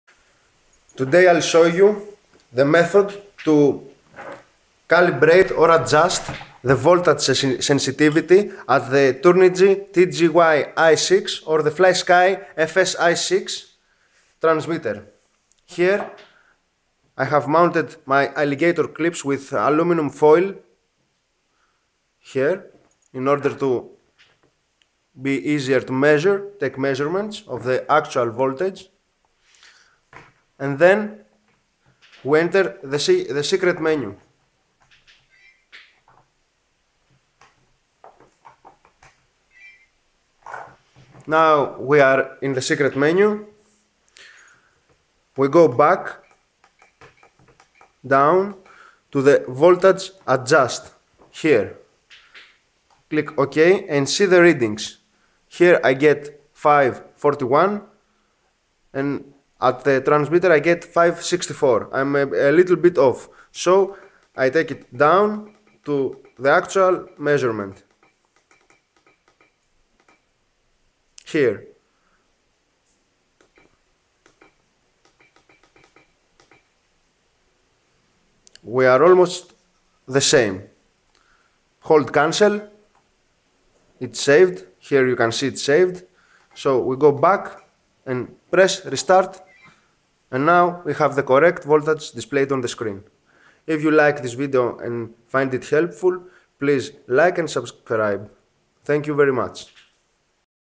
Voltage Sensitivity adjustment calibration on FS-i6 / TGY-i6
This is the method to correctly adjust the voltage sensitivity at your Turnigy / FlySky i6 transmitter. In order to go to the secret-factry menu see this